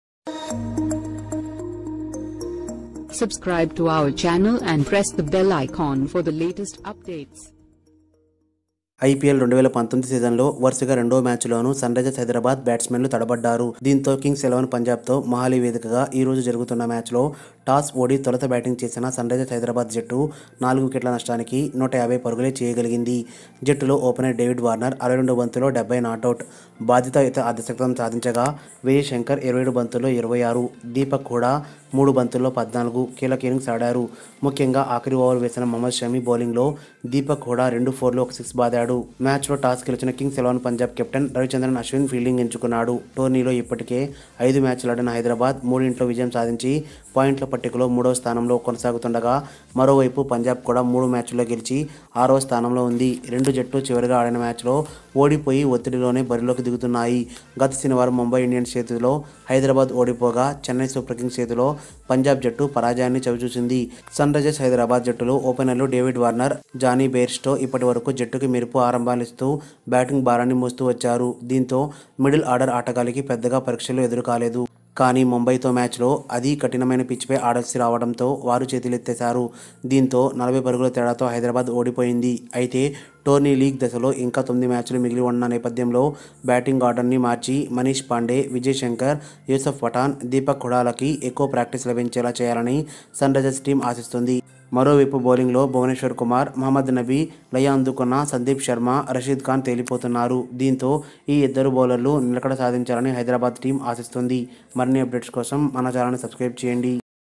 IPL 2019: SRH vs KXIP Full Match Highlights || SRH vs I 2019 Full Match Highlights
Chris Gayle, who had hit a hundred in IPL 2018 in Mohali vs Sunrisers Hyderabad, departs for 16 in their chase of 151. Rashid Khan gets the big wicket for Sunrisers Hyderabad. Earlier in the evening, David Warner's patient 70 helped the visitors post a competitive total on the board.